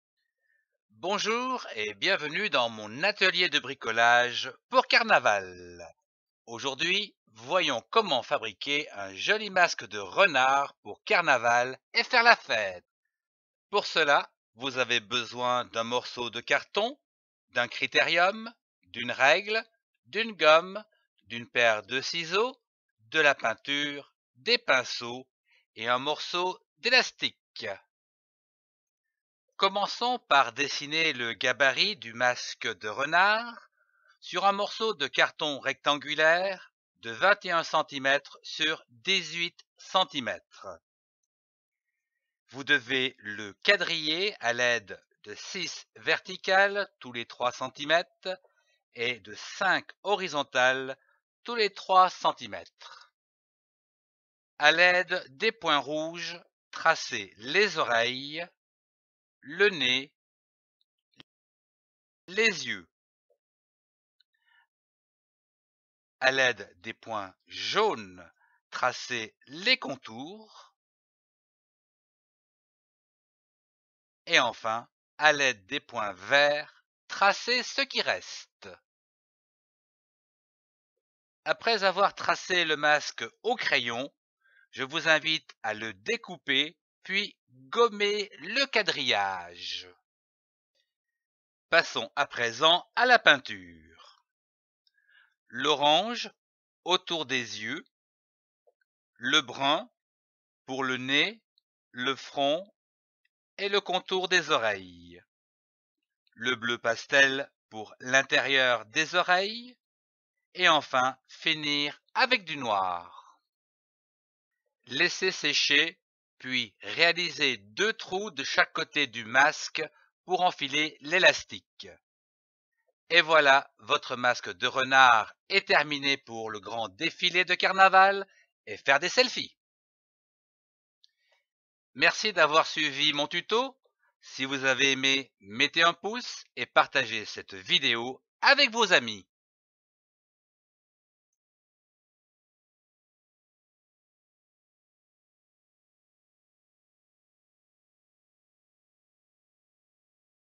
Masque de renard : Fabrication masque d’animaux pour Carnaval
Voici un petit tuto pas à pas pour fabriquer un masque de renard pour carnaval ou toutes autres fêtes déguisées, anniversaire, cosplay, bal masqué, cavalcade, animations, … Facile fabriquer, que vous soyez grand ou petit,  pas besoin d'être doué pour se déguiser et faire de l'effet ... A l’aide d’un gabarit, le masque est facile à réaliser et peut être peint aux couleurs de votre choix. En fin de cette vidéo, retrouvez en un clic mes playlists de déguisement, maquillage et masques de Carnaval pour vous amuser, participer à un cosplay, rigoler et faire de Carnaval une grande fête.